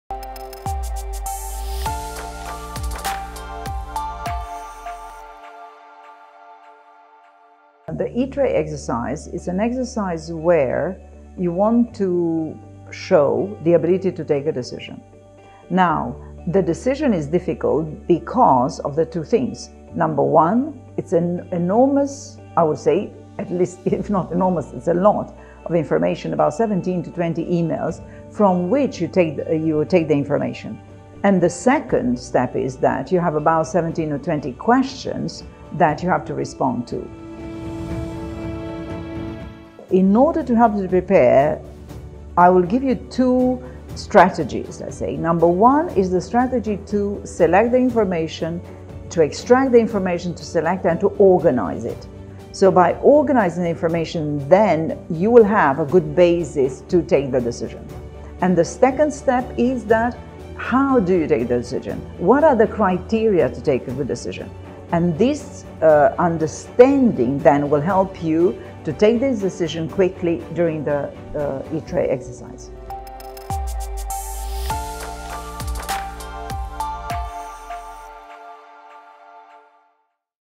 Carmen Peter - EPSO E-tray exercise
Carmen can help you to create strategies to read, organise and respond to the information provided in a timely and effective way in order to support your presented answers at the EPSO E-tray exercise. Find out more about how Carmen can help you on: